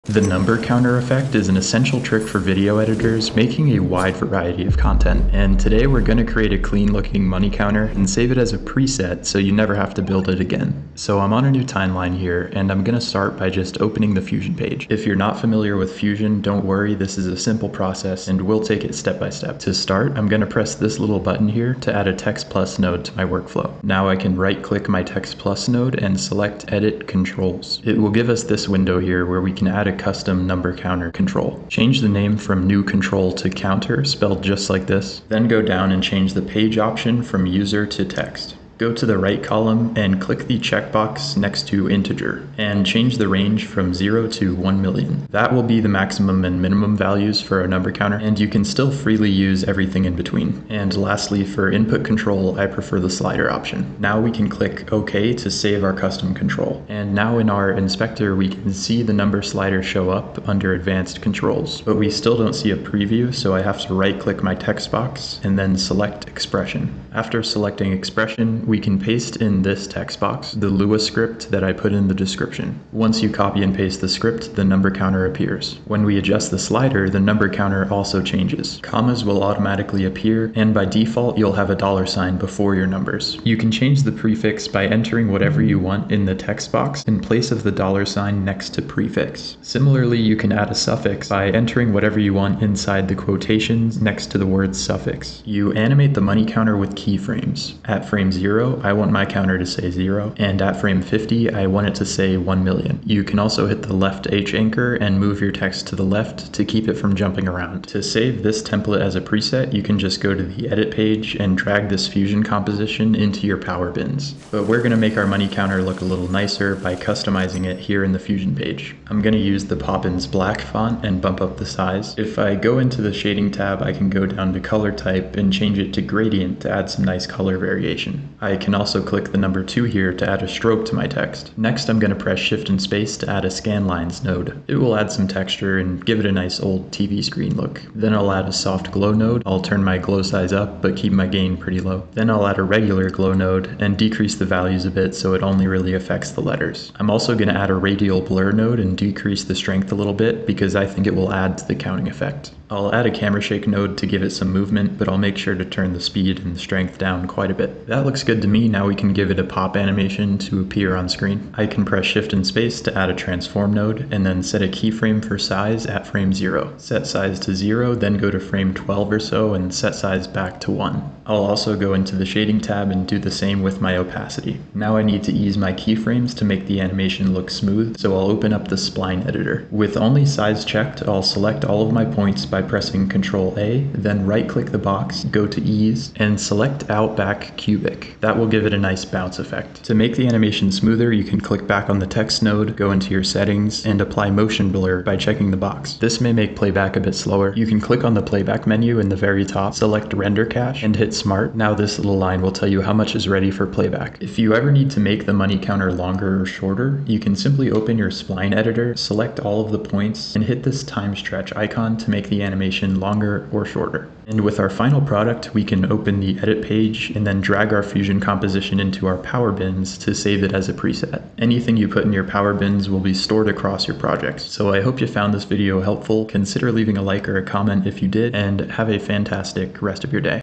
Clean Number Counter Effect Quick and Easy | DaVinci Resolve Tutorial
This video is a quick DaVinci Resolve tutorial on how to create a clean looking number counter effect and save it as a preset to save time editing. We create the effect in the fusion page using a lua script to modify our text then customize the animation using features built into the free version of Davinci Resolve 19.

Here is the script used in the video:

: -- setup
thousandsSeperator = ","
prefix = "$"
suffix = ""
-- setup end
number = Counter
delimited = (string.format('%d', number)):reverse():gsub("(%d%d%d)","%1" .. thousandsSeperator):gsub(",(%-?)$","%1"):reverse()
return(prefix .. delimited .. suffix)